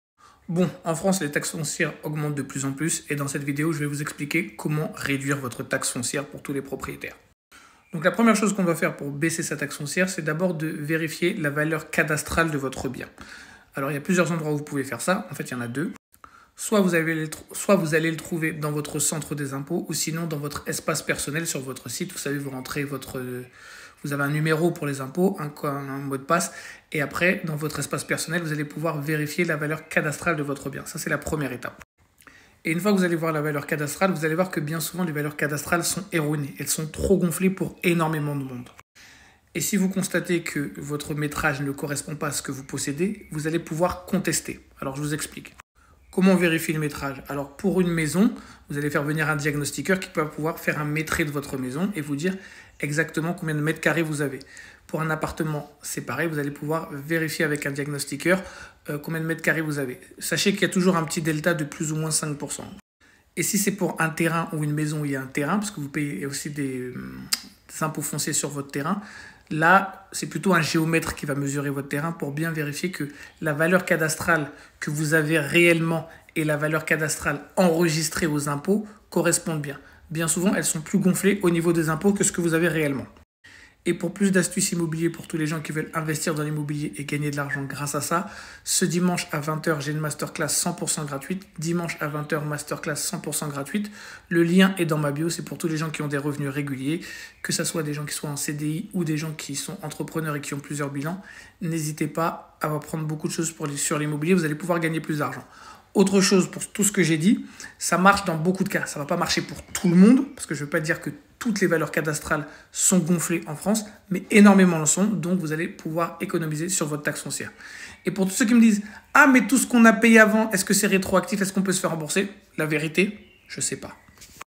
Comment réduire sa taxe foncière
inscris toi à ma masterclass, c'est totalement gratuit

Vous vous demandez comment alléger le fardeau de votre taxe foncière? Ne cherchez pas plus loin! Dans cette vidéo instructive, nous vous présentons une série d'astuces et de conseils pratiques pour réduire efficacement votre taxe foncière.

Que vous soyez propriétaire d'une maison, d'un appartement ou d'un bien immobilier, cette vidéo vous fournira des stratégies éprouvées pour optimiser vos dépenses fiscales tout en respectant la loi. De la déduction des dépenses admissibles à l'utilisation de exemptions et crédits d'impôt, nous couvrons toutes les bases pour vous aider à maximiser vos économies.

Nos experts partageront des informations précieuses sur les différentes mesures que vous pouvez prendre pour réduire votre taxe foncière, que ce soit en rénovant votre propriété, en investissant dans des améliorations écoénergétiques, ou en explorant les programmes de réduction d'impôts disponibles dans votre région.

Ne laissez pas les taxes foncières peser lourdement sur votre budget! Regardez dès maintenant notre vidéo pour découvrir des stratégies simples mais efficaces pour minimiser votre facture fiscale et économiser de l'argent sur le long terme.